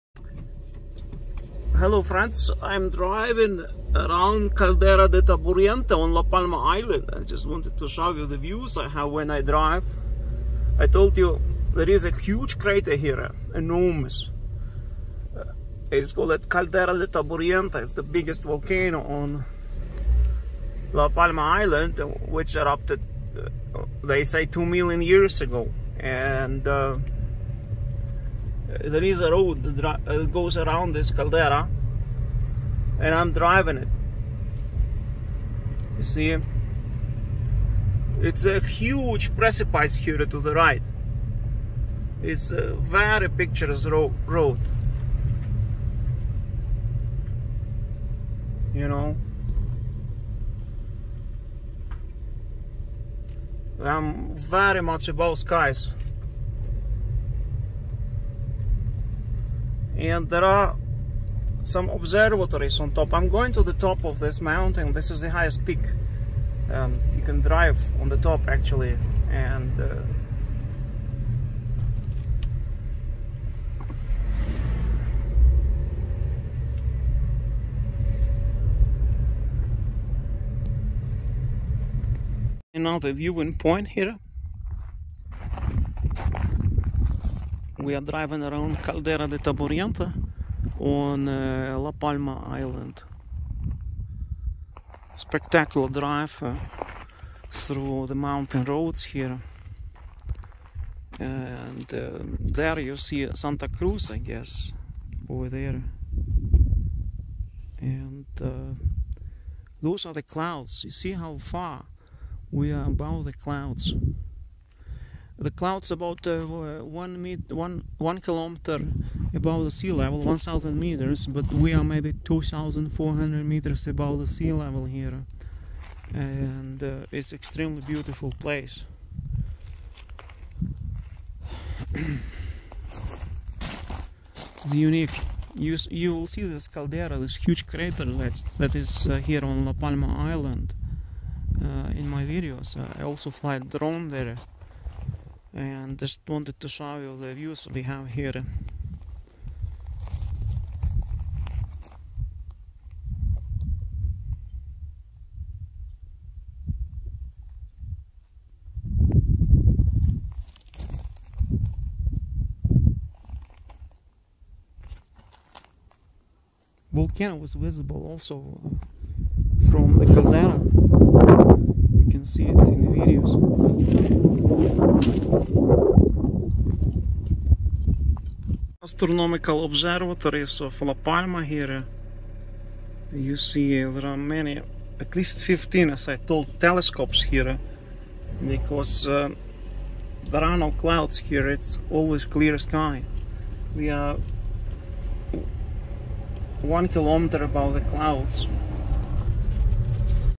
Unknown La Palma: Scenic drive around the Caldera De Taburiente 16.10
Road that goes around the Caldera de Taburiente, on the very summt of La Palma Island - Roque de Los Muchachos. Driving around La Palma on 16.10.21. At Roque de los Muchachos Observatory (ORM), on the rim of the Taburiente National Park, at 2396 metres above sea level in the municipality of Garafía (La Palma) stands one of the largest arrays of telescopes in the world.

Because of the sky above La Palma, this observatory enjoys exceptional conditions for astronomical research, for this reason, it continues to attract giant telescopes of the future, as well as the new generation of Cherenkov telescopes, designed to study the Universe in very high energy gamma rays.

The site currently hosts the largest optical-infrared telescope in the world, along with twenty other telescopes and instruments for various kinds of studies, including nocturnal observations, robotic observing, solar physics, and high energy astrophysics.

Great advances in the study of the Universe have been made with these telescopes, ranging from the detection of the most distant galaxy to confirmation of the existence of black holes and the accelerated expansion of the Universe.

Aside from its scientific activities, the Instituto de Astrofísica de Canarias carries out numerous outreach tasks in an effort to ensure that astronomical knowledge reaches the public at large. For this purpose, at various times of the year school and group visits are arranged to its observing facilities at Roque de los Muchachos Observatory and Teide Observatory.

The Roque de los Muchachos Observatory Residence has a number of facilities (diurnal and nocturnal dormitories, kitchen, dining room, reception, lounges, games, etc.) to fulfil the needs of all the scientific and technical staff link to the Observatory.